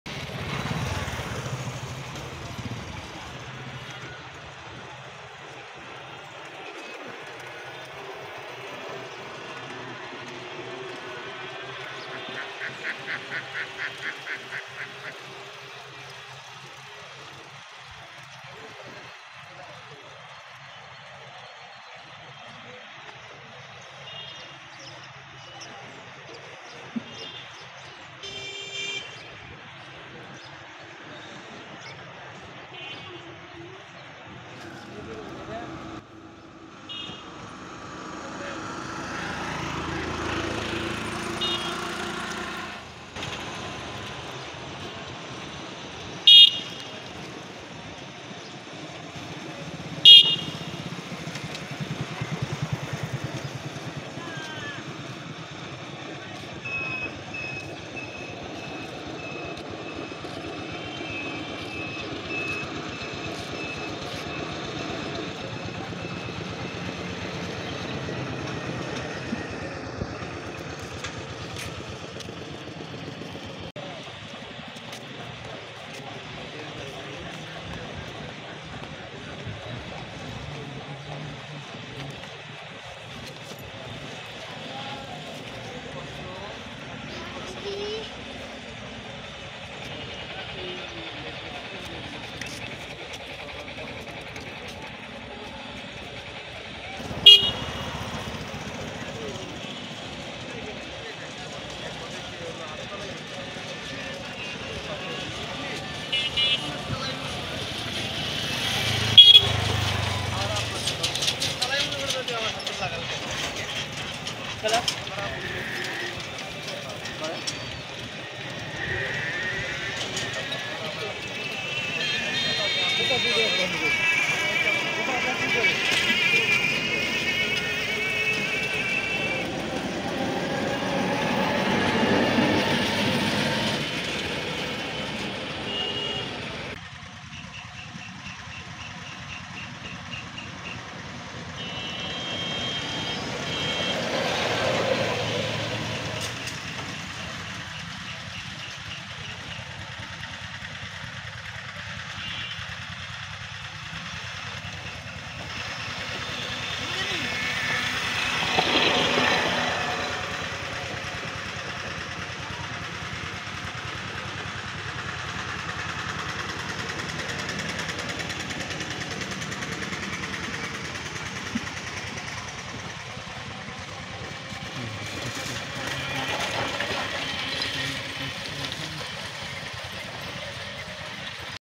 খরা জাল ফেলে মাছ ধরার মজাই আলাদা। এভাবে জাল ফেললেই দেশী মাছের সমারাহ।
The fun of fishing with dry nets is different. If the net is thrown like this, the local fish will be saved.